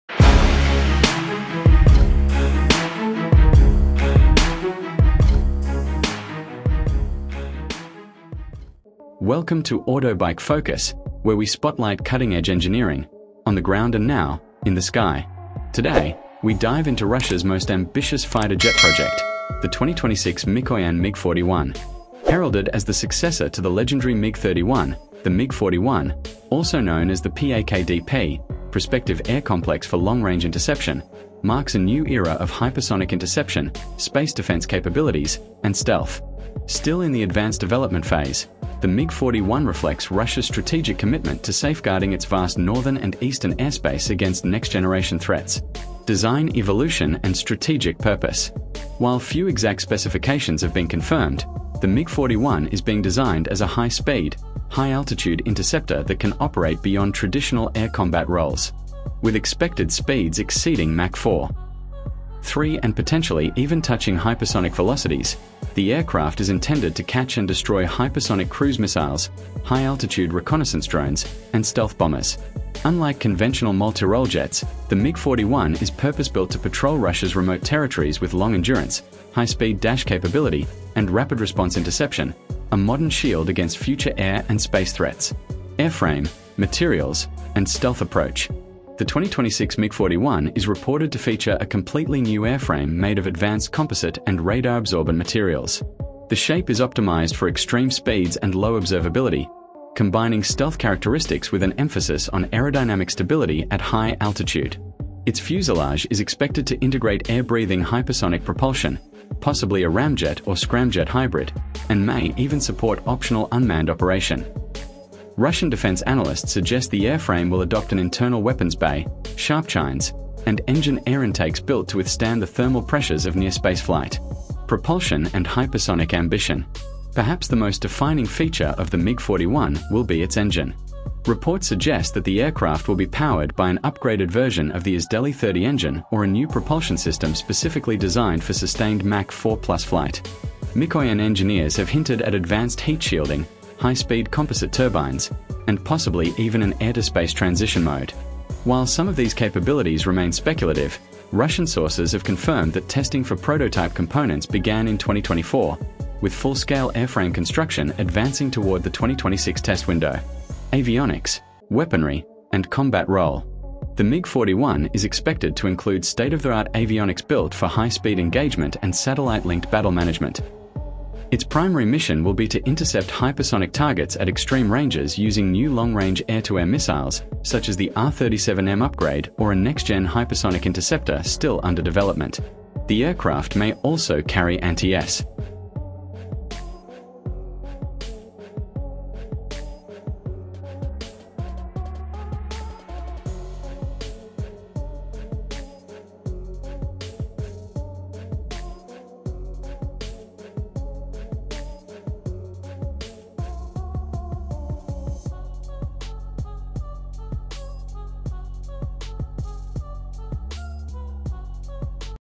The MiG-41 Is Finally Here – Russia's Hypersonic Ghost Fighter Just Changed Everything!
Description:
The legendary MiG-41 has finally been revealed — and it's unlike anything the world has ever seen. Capable of reaching near-space altitudes, equipped with hypersonic speed, stealth tech, and rumored AI integration, the MiG-41 is Russia’s answer to 6th-gen air dominance. Is it real? Can it take on the SR-72 or even intercept ICBMs? In this video, we break down 10 insane facts about the 2026 Mikoyan MiG-41 that will leave you speechless.

⚠️ Don't blink — this could be the start of a new era in aerial warfare.

🔟 Top 10 Segment Titles (for YouTube chapters or thumbnails):

The Hypersonic Monster is Real

Mach 5+ Speed Confirmed?!

Built to Replace the MiG-31

Can It Intercept Missiles in Space?

Stealth Capabilities Like Never Before

AI-Driven Dogfighting?

Plasma Stealth Rumors Explained

Weapons You’ve Never Heard Of

Why NATO Is Worried

MiG-41 vs SR-72 – Who Wins?